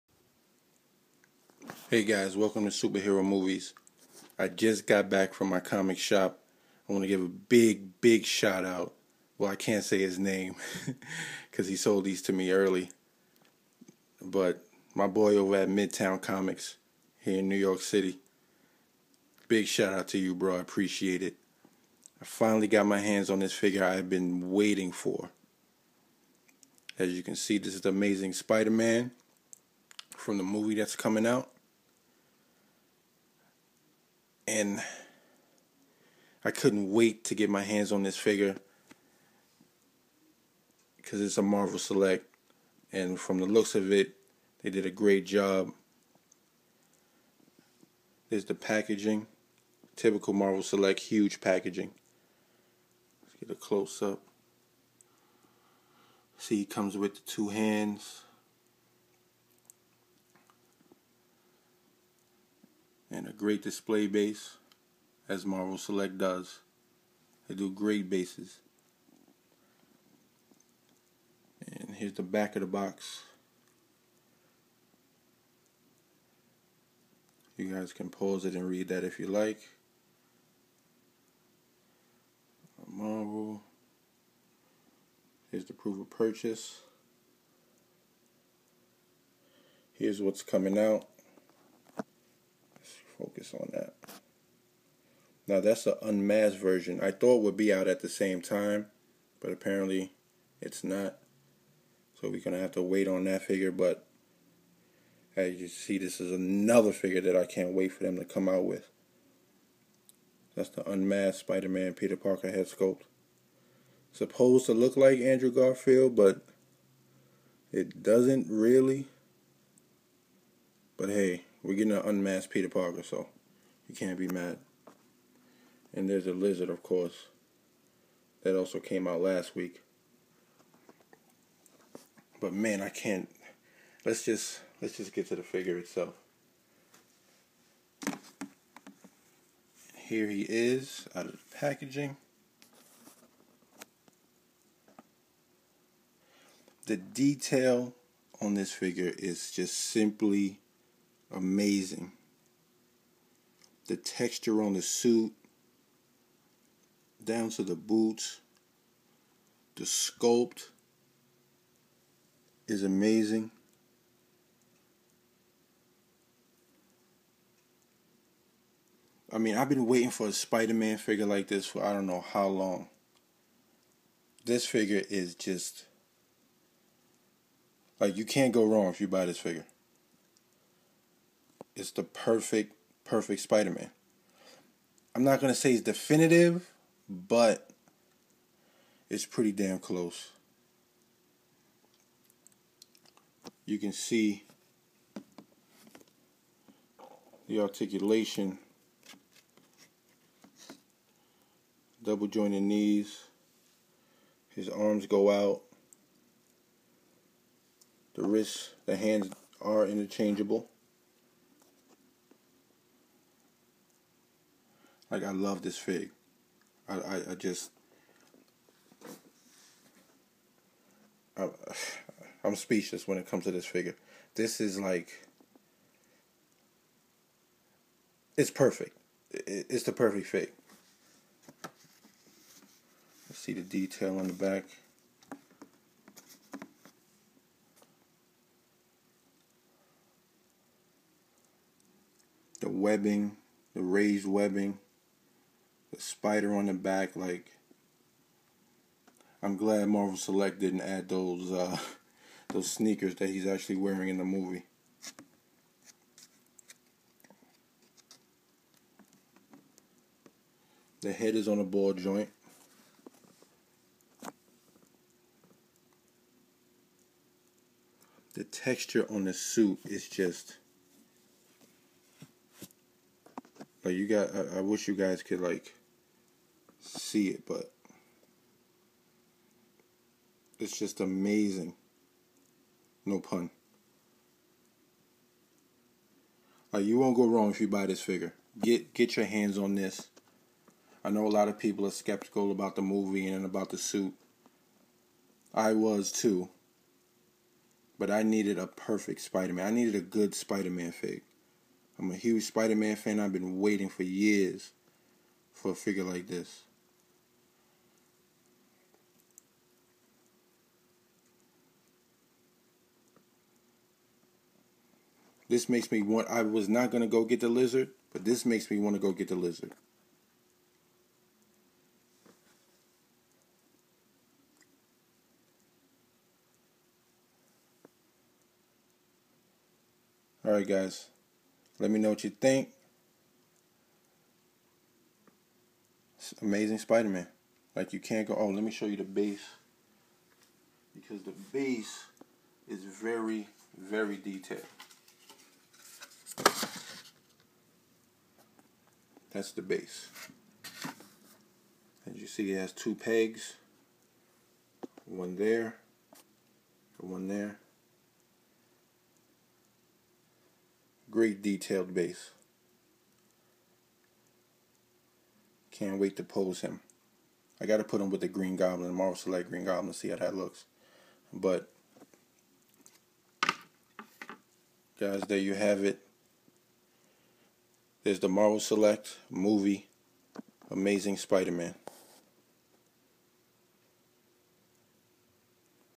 Marvel Select Movie Amazing Spider-Man Action Figure
Review of the Movie Marvel Select Amazing Spider-Man action figure! Thanks to Mid-Town comics in NYC for hooking me up early!! My most Anticipated figure & it doesn't disappoint!! Perfect scale with the Marvel Select Green Goblin..